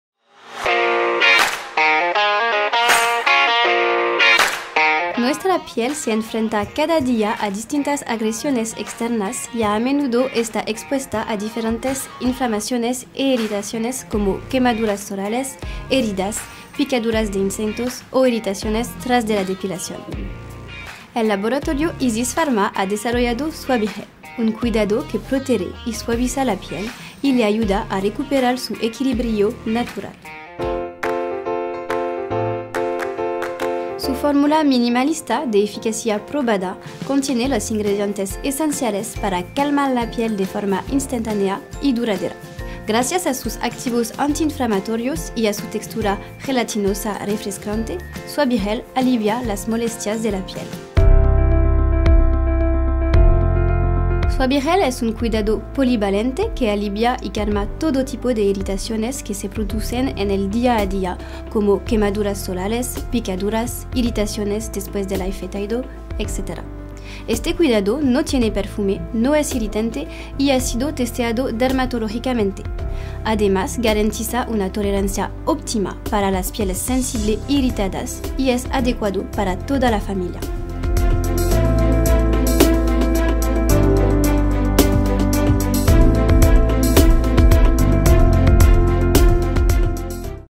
Presentación de la gama Suavigel
Suavigel es una fórmula minimalista y eficaz que contiene Bisabolol y ácido glicirretínico, 2 ingredientes esenciales para calmar la piel de manera inmediata y por un lapso de tiempo largo.

Suavigel calma las molestias cutáneas rápidamente gracias a sus activos calmantes y a su textura en gel con efecto refrescante.

Se trata sobre todo de un producto sin fragancia, no irritante y puesto a prueba bajo control dermatológico, que garantiza una tolerancia óptima en aquellas pieles sensibles e irritadas. ¡Y lo mejor de todo es que es apto para toda la familia!